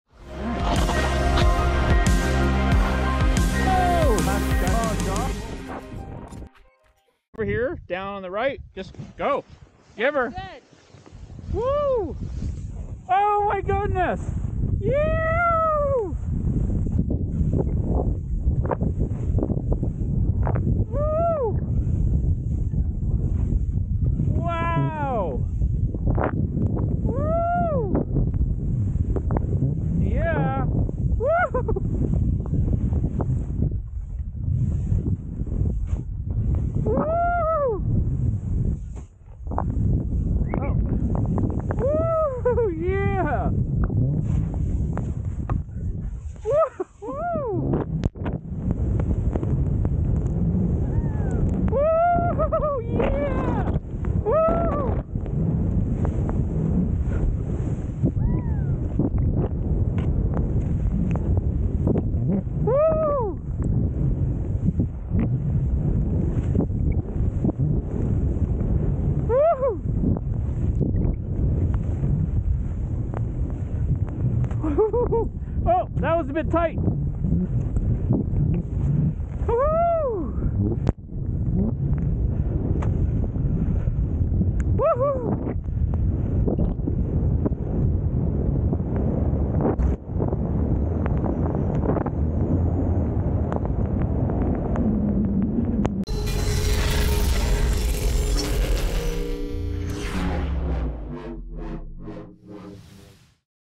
Deep Powder Days on Blackcomb in 8K Poop Chutes onecutmedia
Deep Powder Days on Blackcomb in 8K   Poop Chutes

I think I have only been disappointed a few tomes after making the trek over to the poop chutes on Blackcomb mtn.  It usually like this!

Shot on the Insta360 OneX2

Camera Insta360
OneX2

This summer the photo sessions are back in full swing!
Book your shots soon, days are filling up,